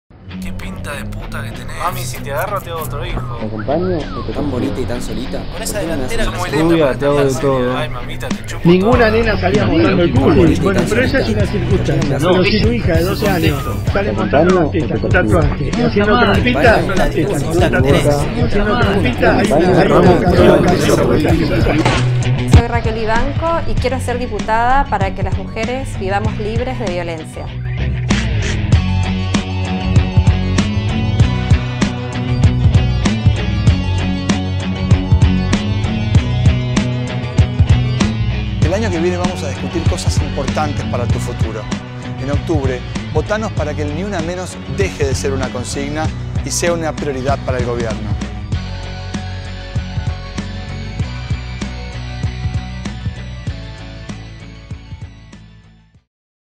Para que las mujeres vivamos libres de violencia
Queremos a Raquel Vivanco y Matías Tombolini en el Congreso

Raquel Vivanco preside el Observatorio de la violencia contra las mujeres NiUnaMenos que presentó recientemente un informe sobre Acoso Callejero y abuso en el transporte público.
Dicho estudio arroja que el 93% de las mujeres encuestadas en 11 provincias dijo haber sufrido acoso callejero desde edades tempranas.
Por eso convertir en Ley la sanción del Acoso Callejero es una de nuestras propuestas